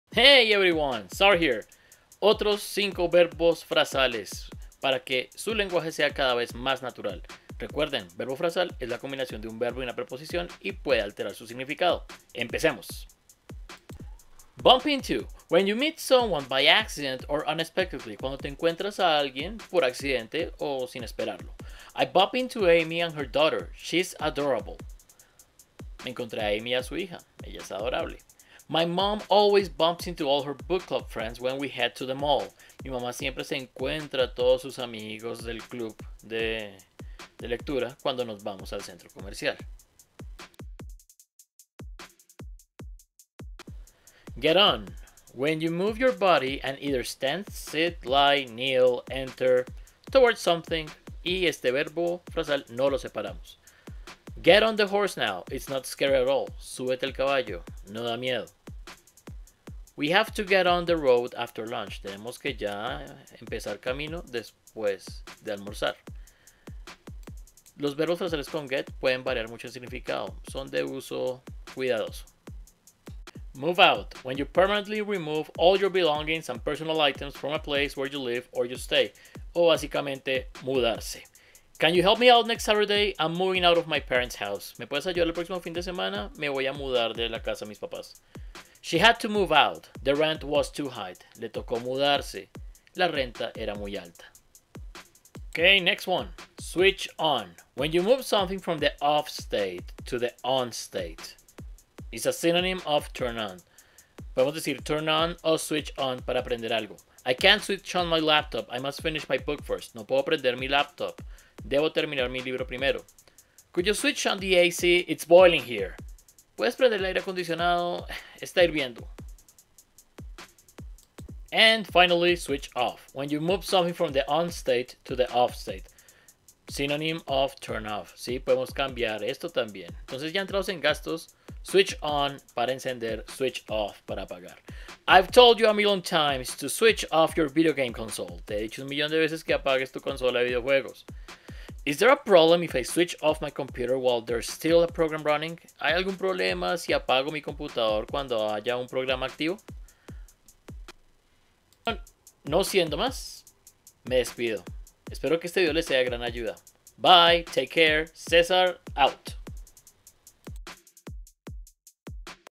5 Phrasal Verbs in Español #3
💁🏾‍♀️ Phrasal Verbs! Otros 5 verbos frasales para integrar a su discurso diario.
Lo sé, esta serie va a ser larga, pero si aprenden 10 expresiones a la semana y las usan, se que mejorará su nivel de forma estable 🤓

0:00 Intro
0:14 Bump into
0:43 Get on
1:13 Move out
1:35 Switch on
2:01 Switch off